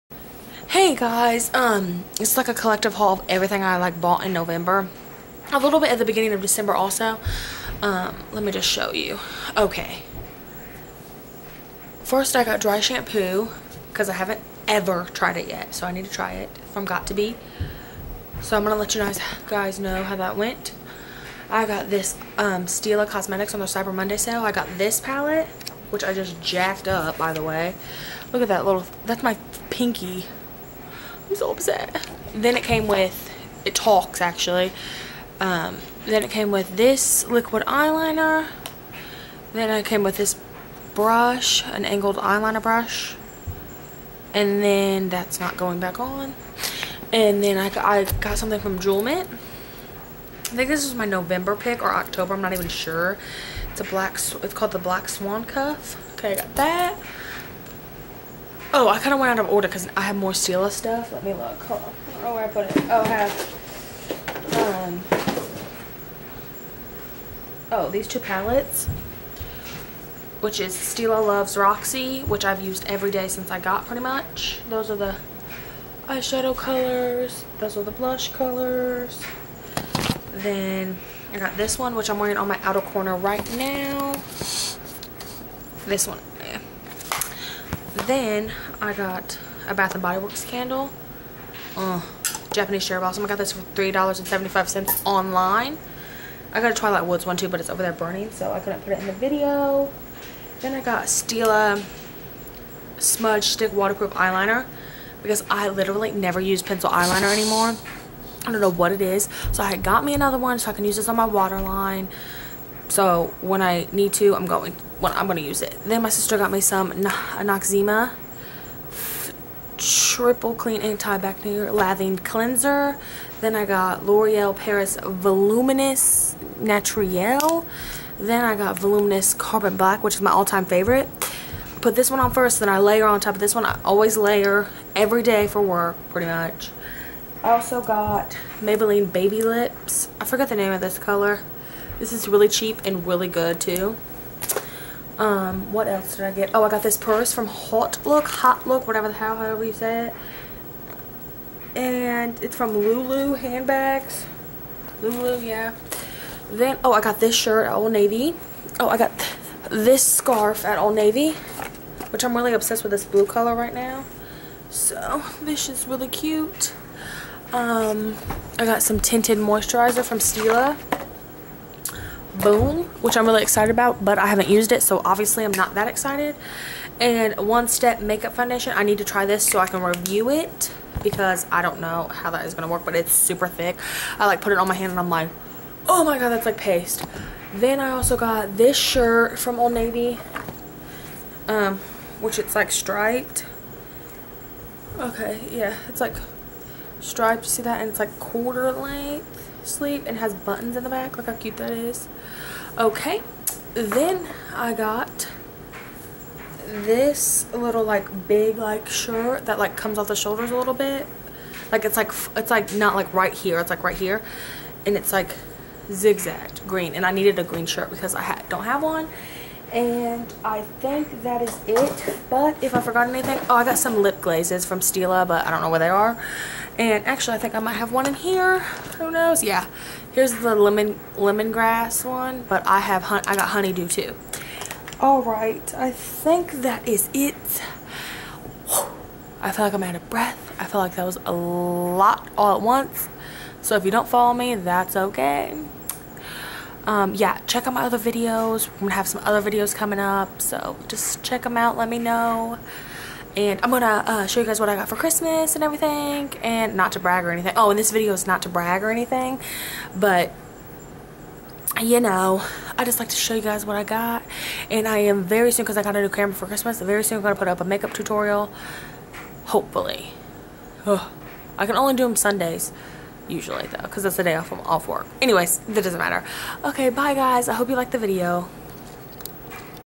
Stila , Old Navy , and Drugstore Haul
This is basically everything i bought in november and december. Its a collective haul.